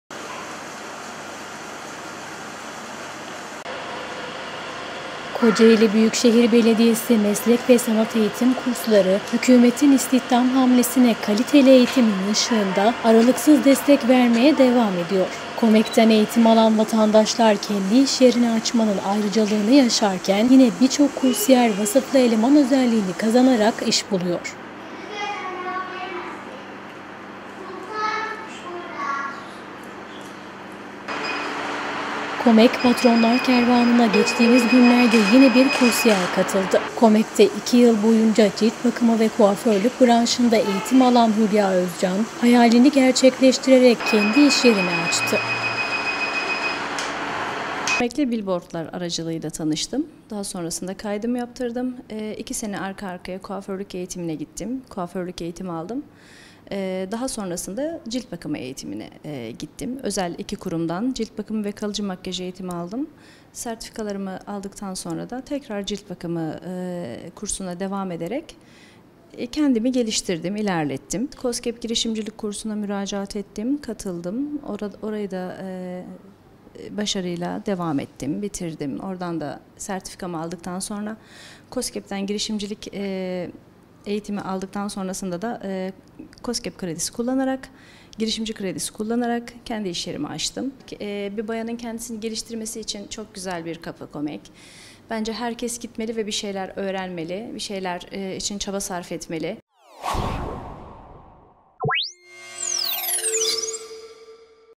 KO-MEK’le kendi işinin patronu oldu
KO-MEK’ten eğitim alan vatandaşlar kendi iş yerini açmanın ayrıcalığını yaşarken, yine birçok kursiyer “vasıflı eleman” özelliğini kazanarak iş buluyor. KO-MEK Patronlar Kervanı’na geçtiğimiz günlerde yine bir kursiyer katıldı. KO-MEK’te iki yıl boyunca Cilt Bakımı ve Kuaförlük branşında eğitim alan Hülya Özcan, “hayalini” gerçekleştirerek, kendi iş yerini açtı.